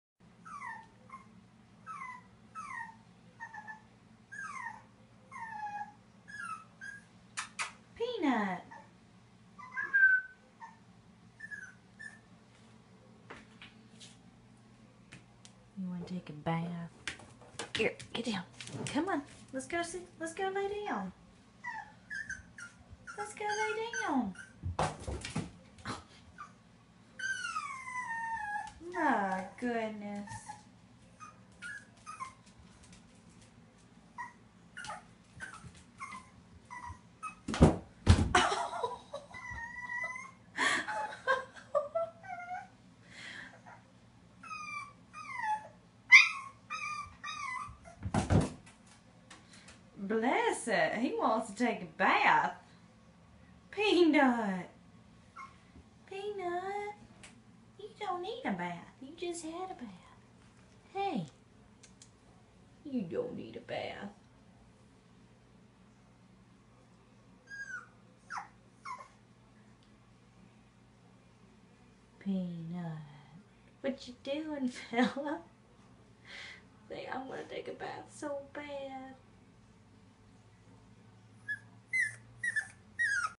Puppy dog whimpers and crashes into bathtub because he wa...
A black and white Boston Terrier puppy dog stands outside a large empty bathtub and whimpers because he wants to take a bath. He runs at the tub, crashes, and falls on the floor at 00:30:00, then continues to jump up the step and look into the tub.